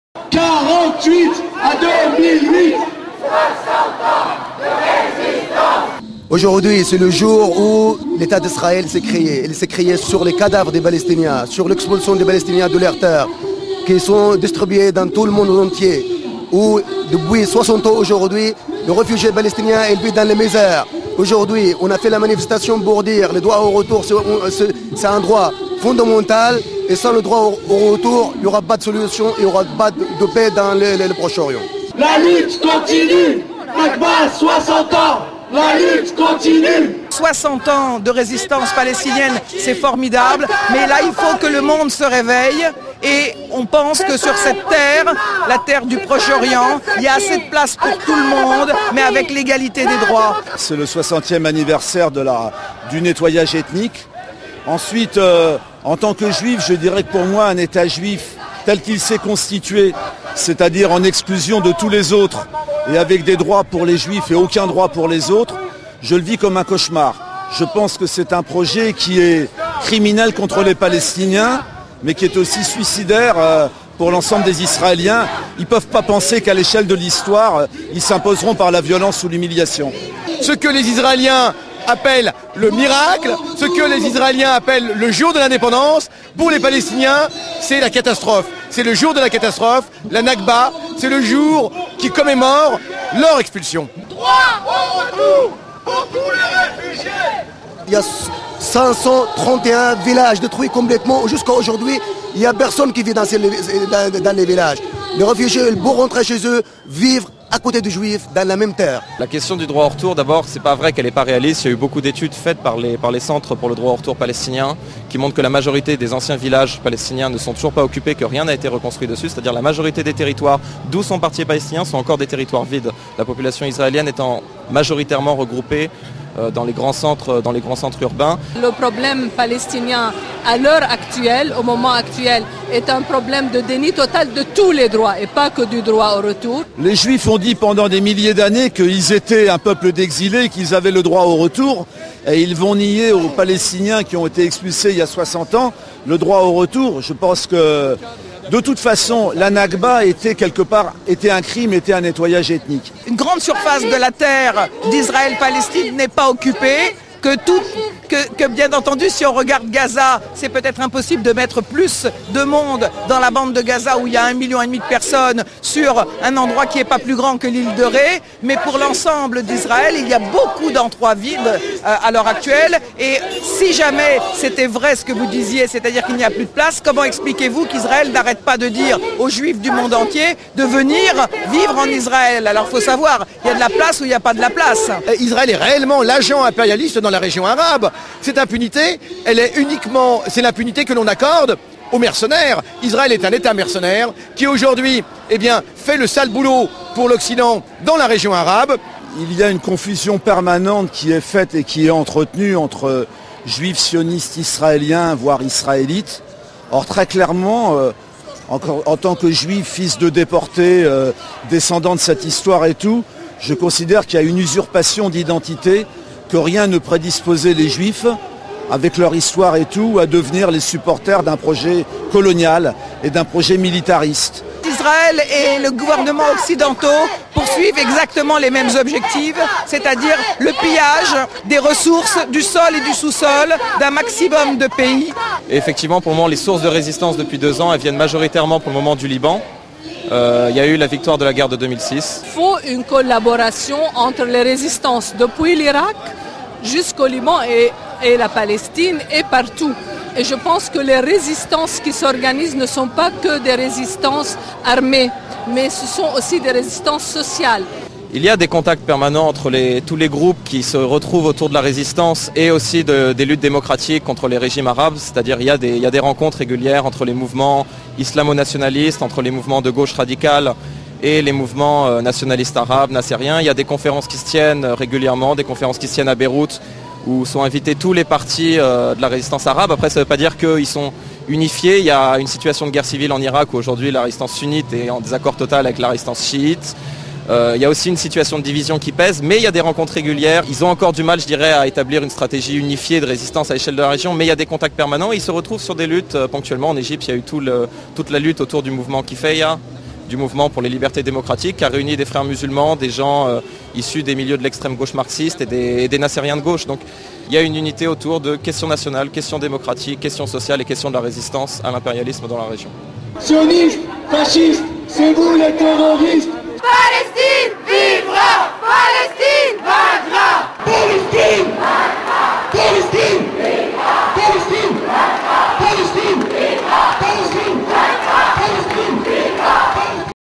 Le Droit au Retour des Réfugiés Palestiniens
Manifestation à Paris le 17 mai 2008 pour le droit au retour des réfugiés palestiniens expulsés depuis 1948 par Israel. A l'occasion du 60ème anniversaire d'Al-Nakba (14 mai 1948).